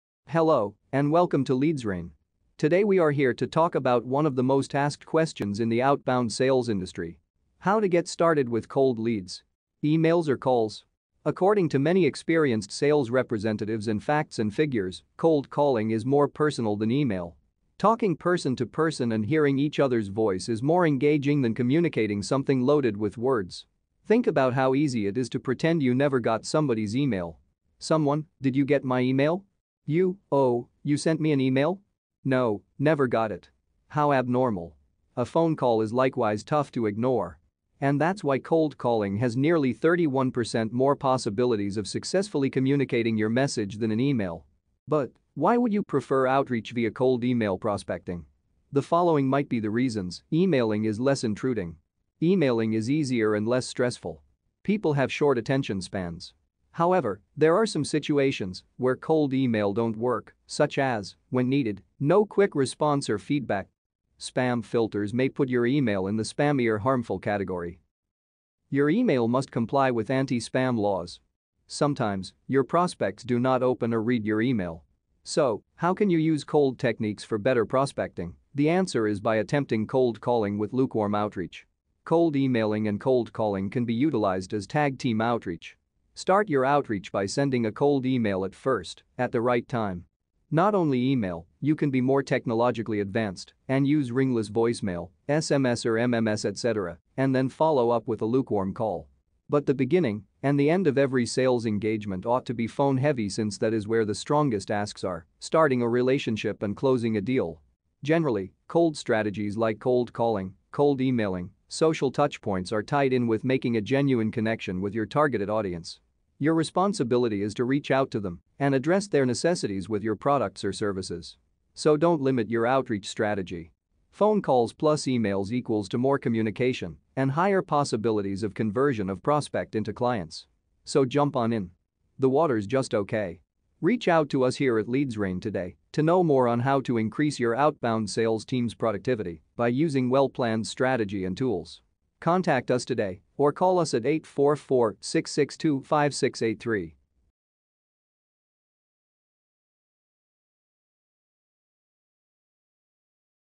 Cold Email vs Cold Calling: Which one to elect
Call or Email is always a dilemma when you are thinking of reaching your cold leads. Choosing anyone for outreach is a smart decision to make, that works better in which situation.